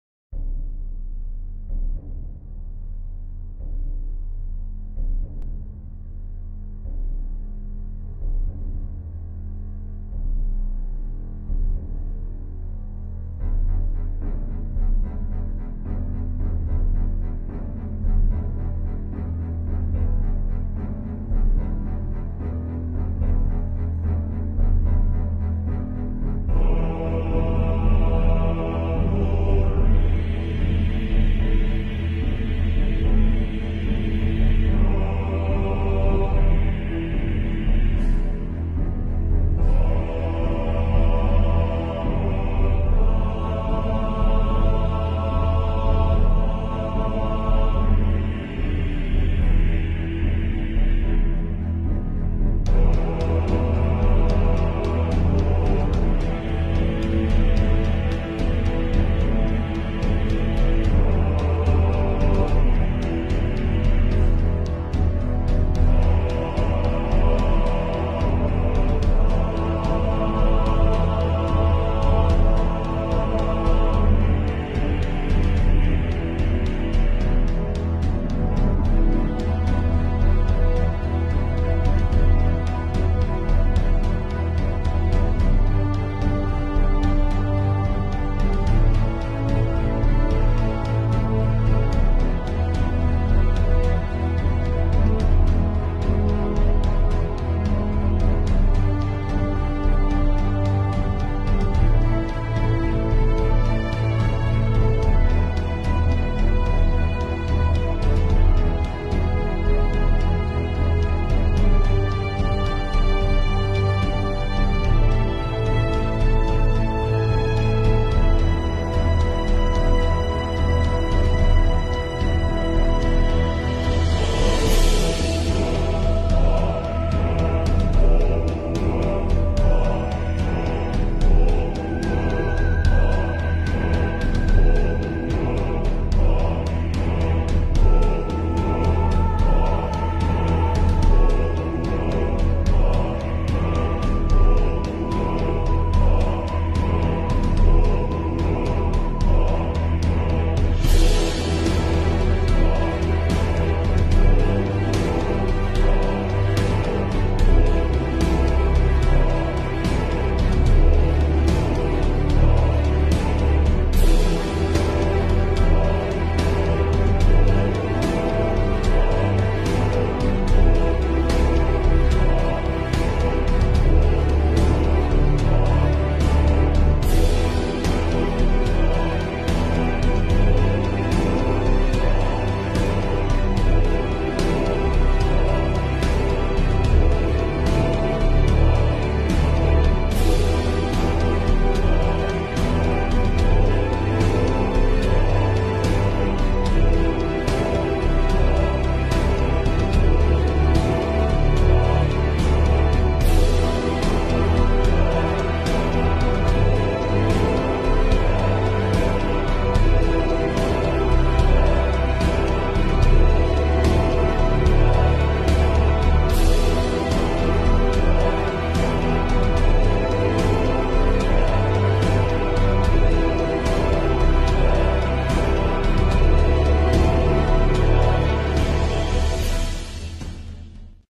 Viking Character | Digital Painting | Character Design Timelapse
A Barbarian character I had been working on, wanted to practice a little more anatomy since it has always been a hard topic for me.